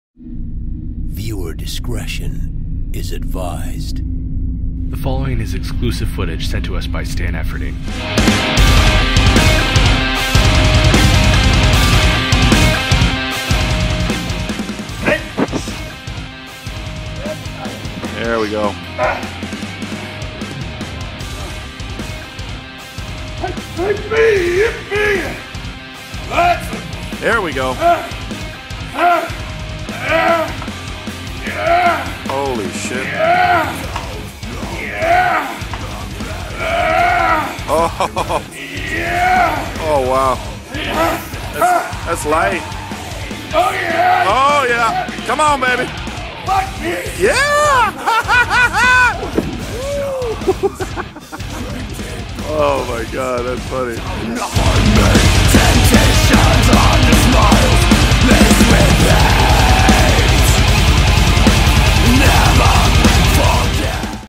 Stan RHINO Efferding 210 lbs Incline Dumbbell Presses || RATED R
Stan RHINO Efferding breaks his record and hits the 210 lbs dumbbells for 10 reps on Incline Presses!!

Thanks to Gabe Moen for filming this EPIC event!